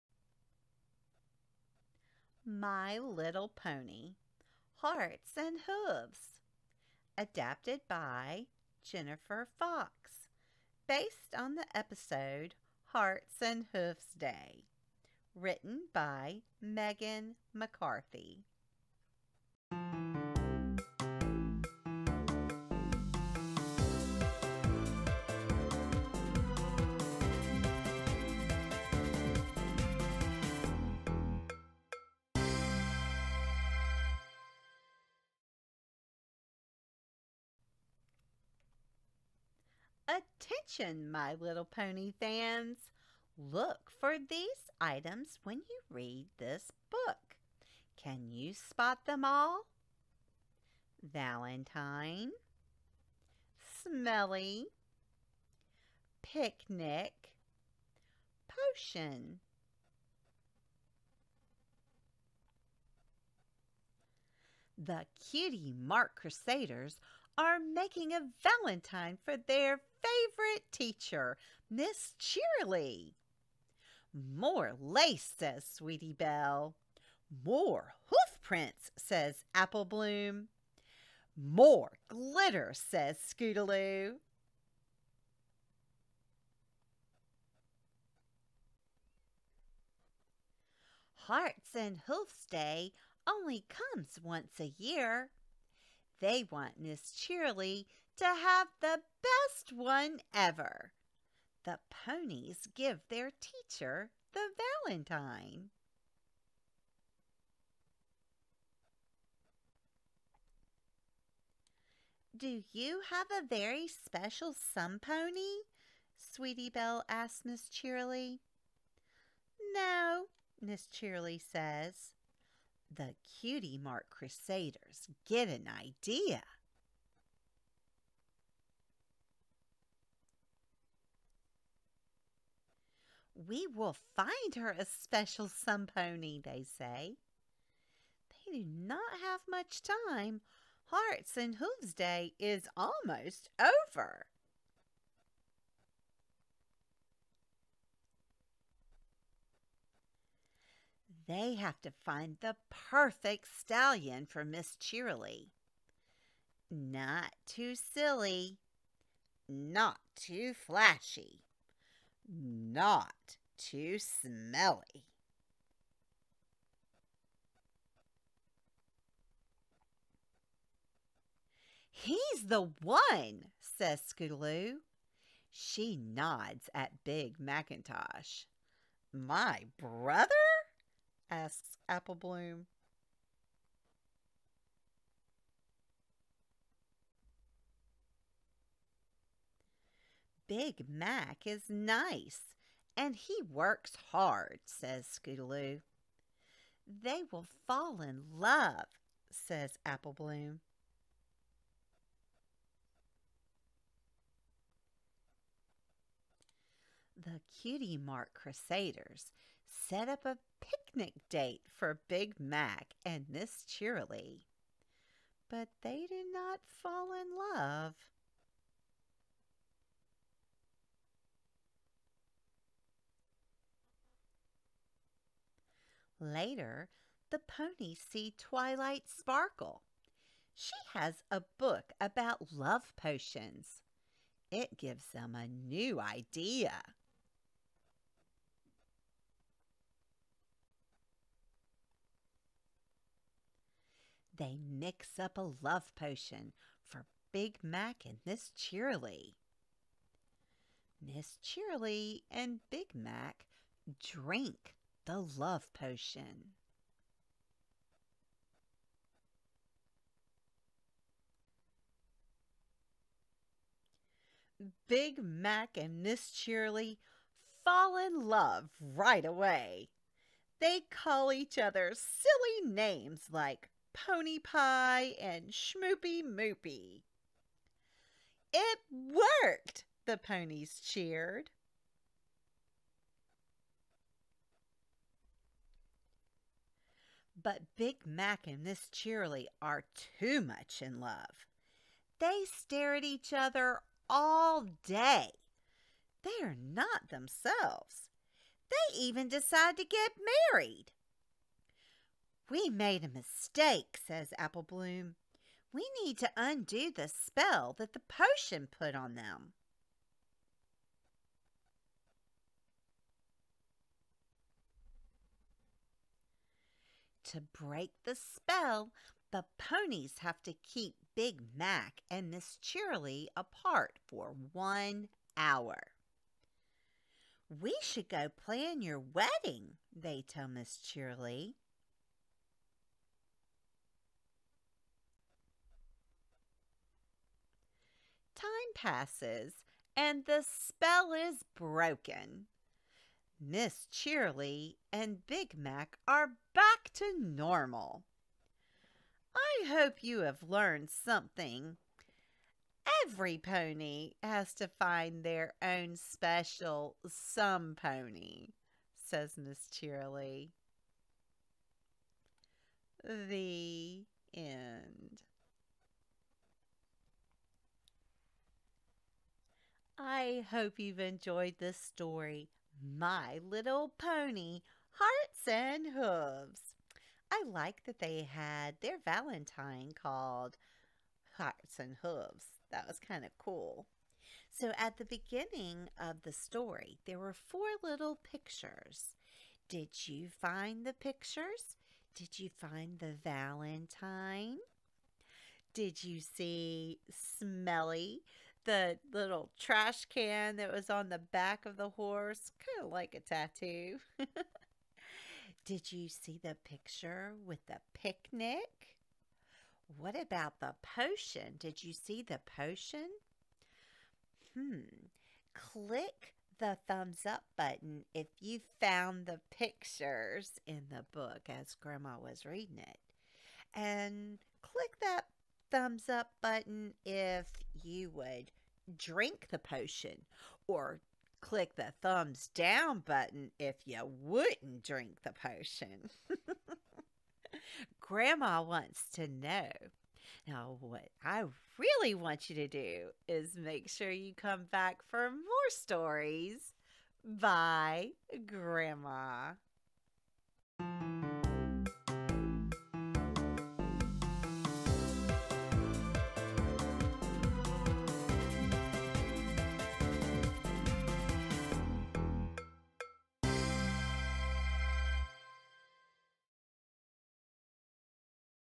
Valentine's Story~ MY LITTLE PONY HEARTS AND HOOVES Read Aloud ~ Story Time ~ Bedtime Story
Follow along with Grandma as she reads aloud My Little Pony Hearts and Hooves!  The Cutie Mark Crusaders are so excited to celebrate Hearts and Hooves day! They go on a mission to find a special Valentine for their teacher, Cheerilee, but soon realize they are in big trouble when their love potion spell misfires! Based on the fan favorite television show Friendship is Magic.

Gma